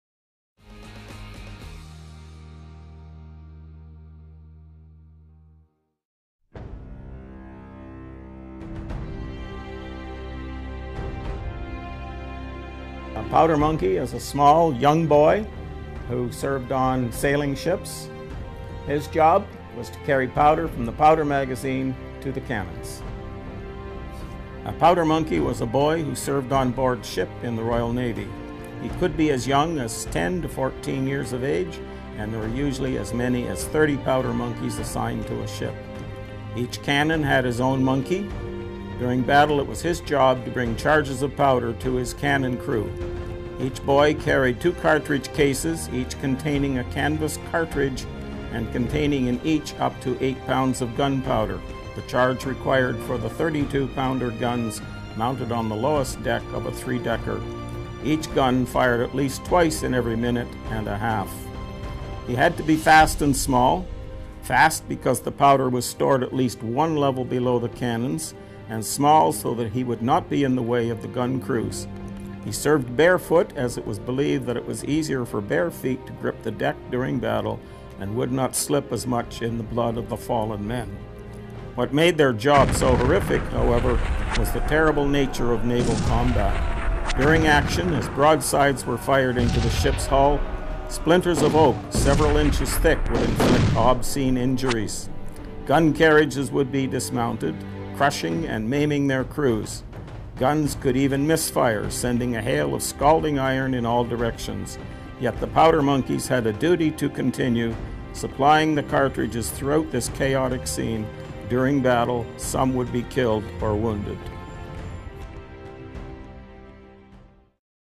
1812 Hamilton - Powder Monkey
'Powder Monkeys' served on board ships in the Royal Navy during the War of 1812. Were there real monkeys on board the ships?